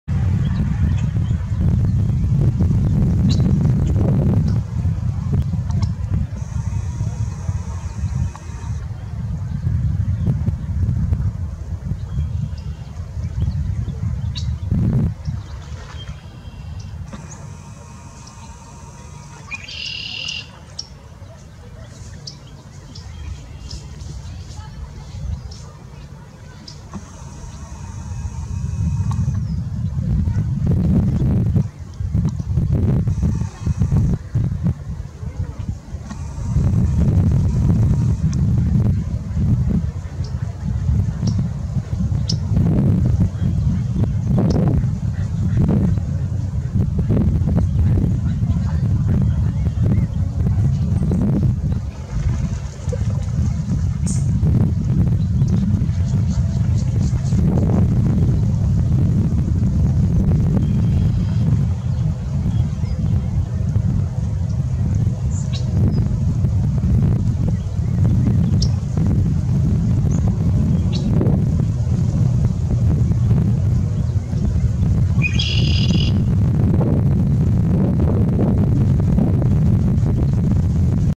Red-Breasted Merganser With Fish
Horrible video, my camera just wouldn’t stay focused. After awhile I said the hell with it and took photos instead. Regardless, enjoy!
Located in Southeast Wisconsin